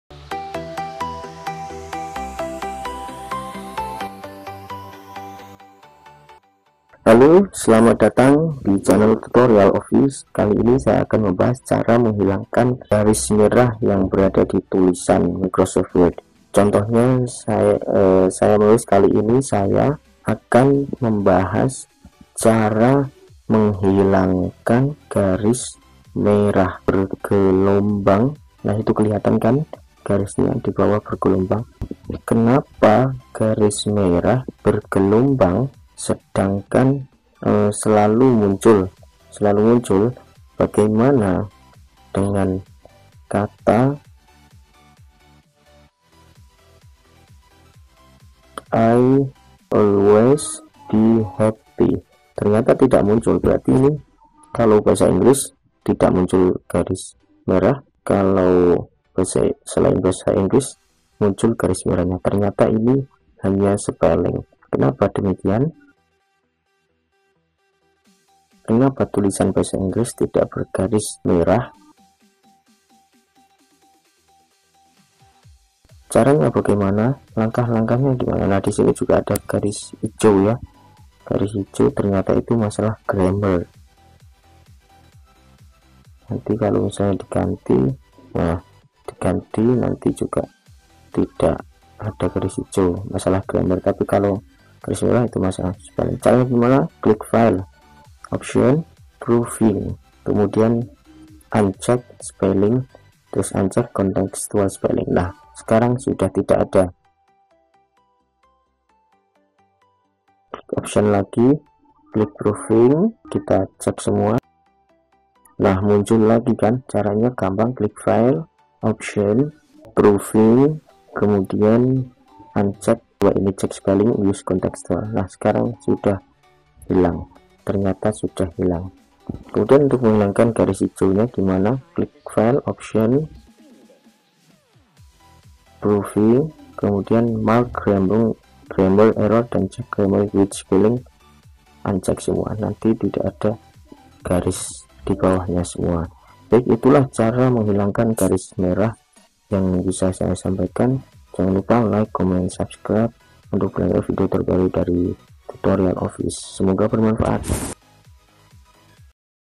Cara Menghilangkan Garis Merah di Word Dan Hijau Dibawah Tulisan
Biasanya ketika kita menulis di lembar kerja microsoft word muncul garis merah bergelombang, terkadang garis tersebut mengganggu konsentrasi kita dalam menulis,tapi tenang saja karena didalam video ini akan kami bahas cara menghilangkan garis merah tersebut.

Cara ini bisa digunakan untuk microsoft word berbagai versi seperti 2007, 2010,2013 dan 2016. Selain itu juga untuk windows 7, 8 dan 10.

Penasaran dengan cara menghilangkan garis merah di microsoft word saat mengetik ? silahkan simak videonya sampai selesai.

Gabung Grup FB Bimbingan Microsoft Office